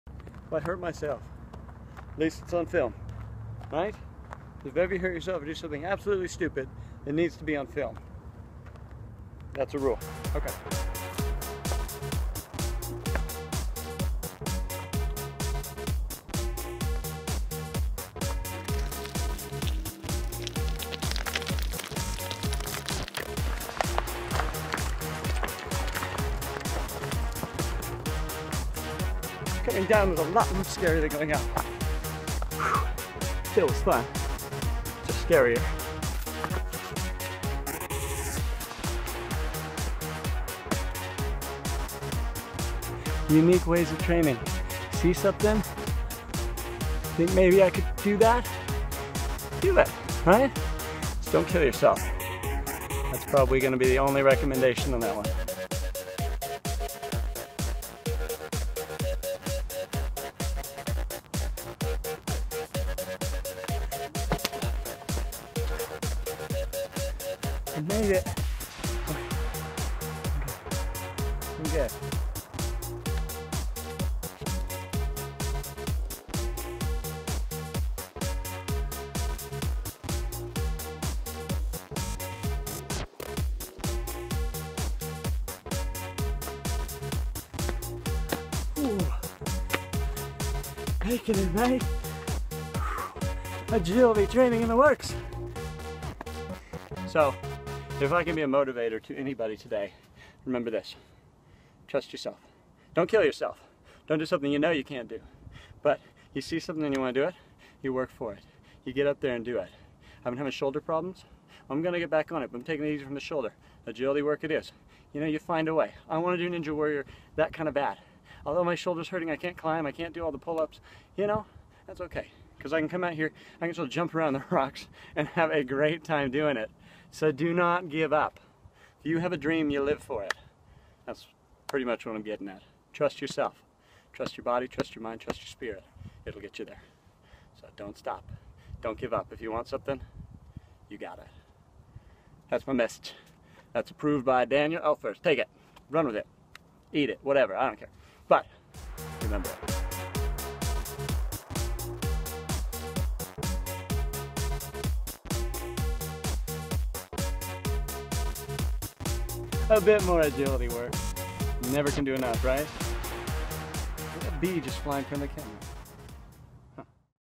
Ninja Training: Agility Rocks
I took myself out to the desert rocks this last week! I've been having some shoulder pains and unfortunately can't climb for 2 weeks so I found a new way to train. Agility is just as important for a Ninja Warrior as the upperbody strength and I definitely can't ignore it. So while I can't climb I'm on my feet more!

Plus I hope an inspirational message for you towards the end! Go live your dreams!

˃˃˃ And if your a Tri-Cities Washington local who loves Ninja Warrior or wants to get connected in check out (site still under construction. Another projects I'm working on!)